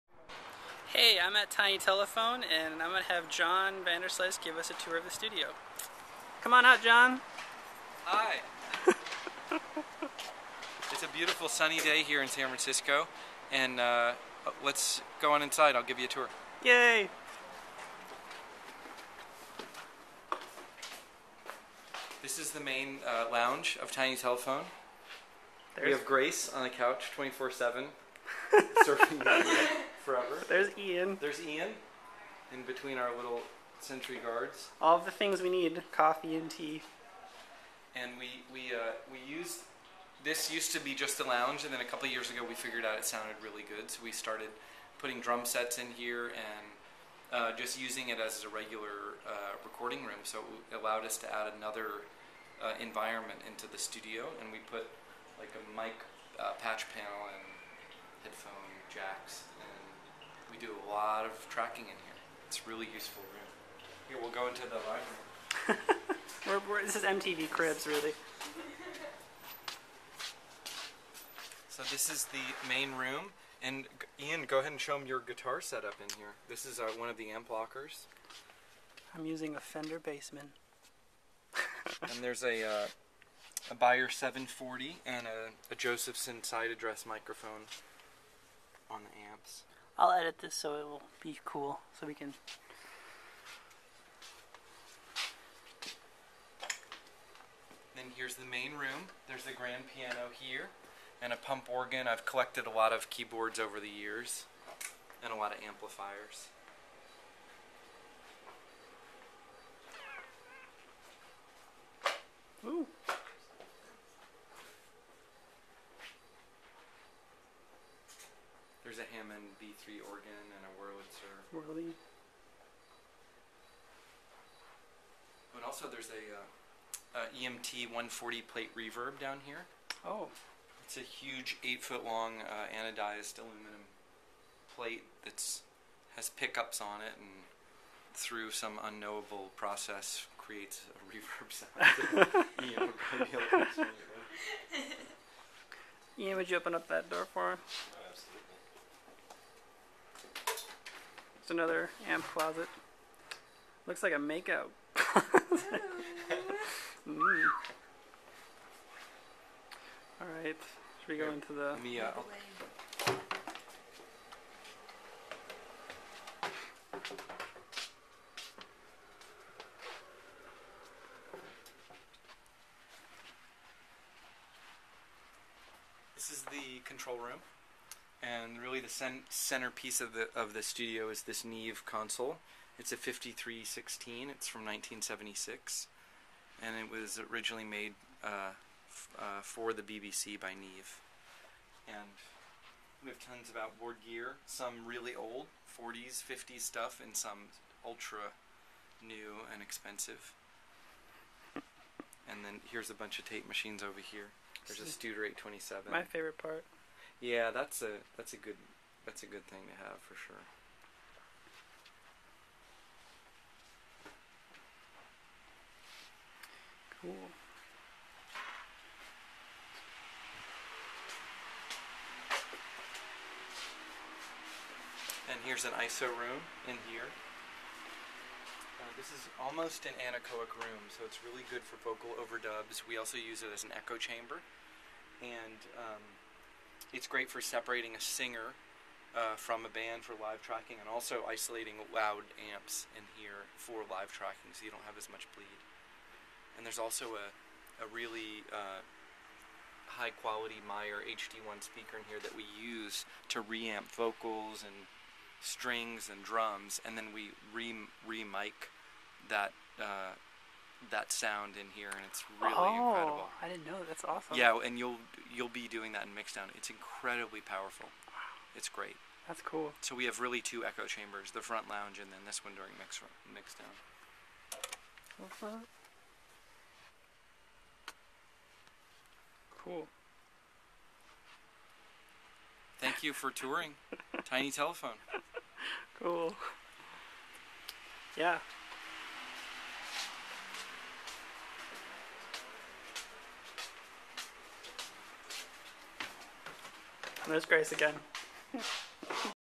One for the Team - Tour of Tiny Telephone Studios with John Vanderslice
Ian Anderson goes on a tour of Tiny Telephone Studios with John Vanderslice in San Francisco, CA. Featuring Grace Fiddler (delightful band member) and Ian Pellicci (delightful engineer). 8/14/09.